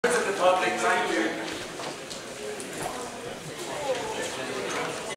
Extraordinary (Wirral Council) Celtic Manor Resort Climate Emergency 15th July 2019 Part 4 of 4
Council Chamber, Wallasey Town Hall, Brighton Street, Seacombe, CH44 8ED

The agenda and reports for this meeting can be found

Continues from Extraordinary (Wirral Council) Celtic Manor Resort Climate Emergency 15th July 2019 Part 3 of 4

4. THE FUTURE OF CELTIC MANOR 0:01

4a Requisition Notice

In accordance with Standing Order 1(4) a request to the Civic Mayor from 22 Members of the Council was received on 24 May 2019to discuss the future of the Celtic Manor Resort. See attached.

Additional documents:

Extraordinary Council - Item 4 Motions , item 4a